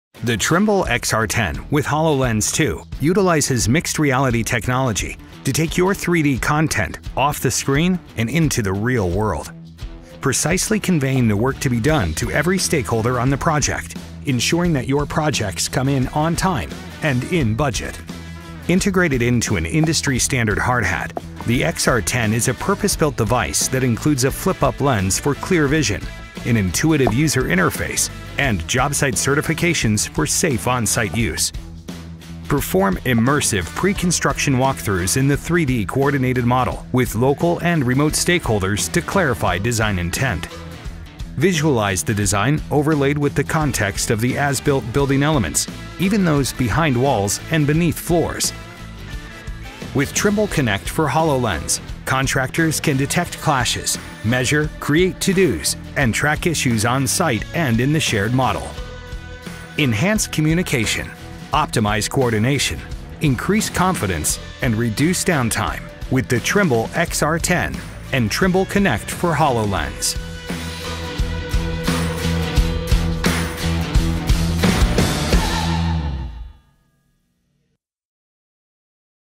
트림블 XR10 with 홀로렌즈2
MS사의 홀로렌즈2가 부착되어 출시된 건축/건설용 VR 장비인 트림블 XR10 소개 영상
(자막설정: 한국어)